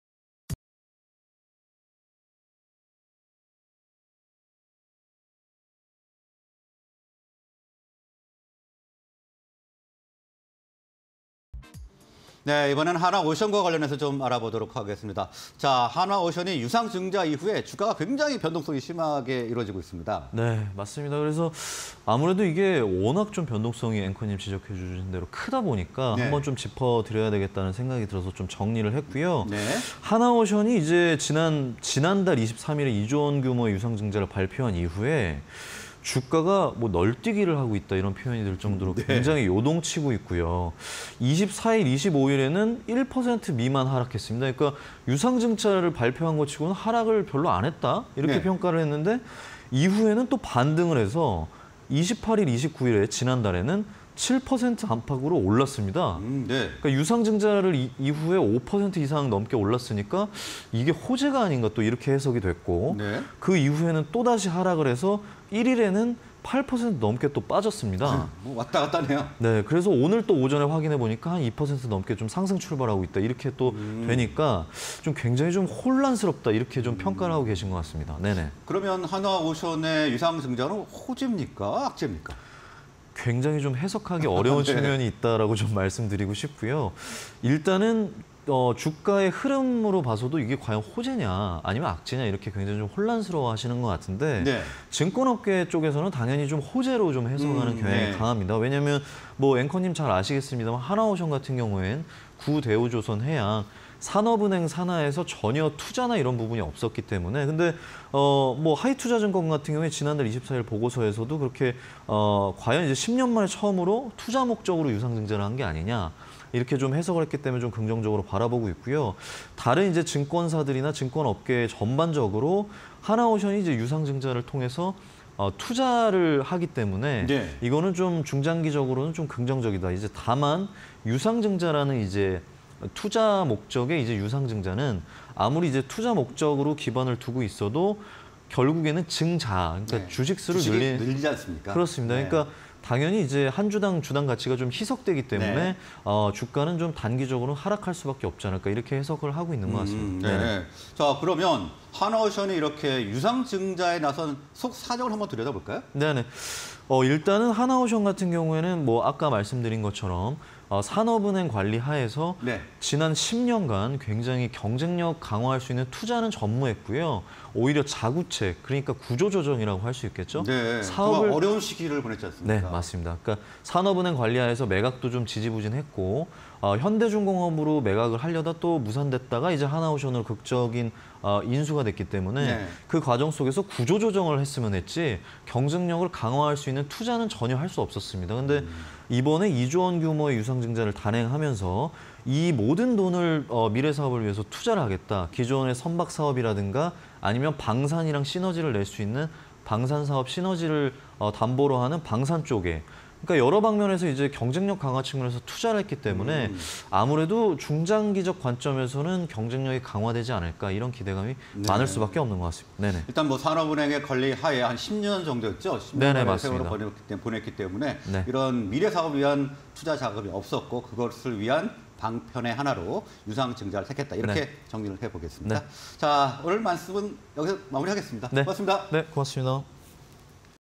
"악재와 호재 사이"... 한화오션 유상증자 의미는?_기자들의 백브리핑 (20230904)
Q. 한화오션, '유상증자 쇼크' 이후 주가 널뛰기?
Q. 호재? 악재?… 한화오션 유상증자에 쏠린 눈
Q. 한화오션, 2조원 유증… "글로벌 해양방산 정조준"
Q. 한화오션, 통근 유상증자 결단… 속사정은?